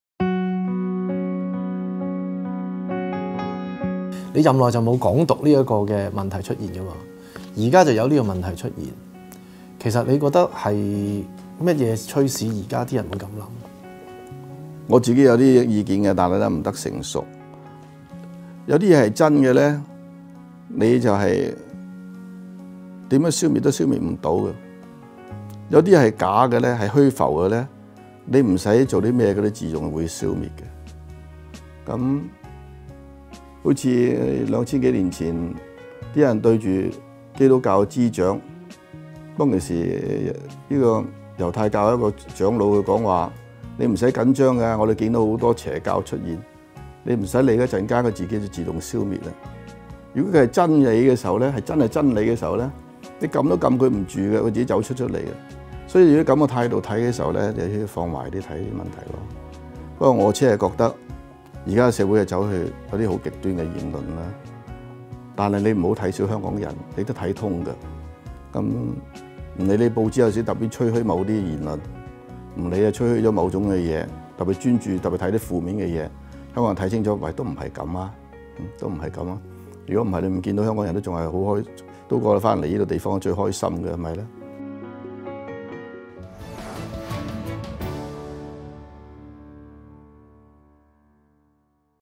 【獨家專訪重溫】 曾蔭權談港獨: 「真的消滅唔到 假的會自動消滅」 2016-09-07
提到港獨思潮時，曾蔭權指自己有些個人看法，但不成熟，他無具體說明想法，僅說：「有啲嘢係真嘅呢，你點消滅都消滅唔到嘅；有啲嘢係假嘅、係虛浮嘅呢，你唔使做咩佢都會自動消滅。」又舉例如二千多年前基督教滋長，猶太教長老指不用理會，邪教會自動消滅，「如果真係真理嘅時候，你撳都撳佢唔住，佢自己走出嚟，所以咁嘅態度睇，放懷啲睇呢個問題」。

他認為香港是聰明人，會做好選擇，「唔好批評佢話會走去某個位置，如果(去到)某個位置發覺對長遠利益唔得好時，佢自己會走番返個位置，我諗最緊要係，任何人都要對呢個土地啲人，不止要投入去，要相信佢，一定會做番好。」

bmgproductionmusic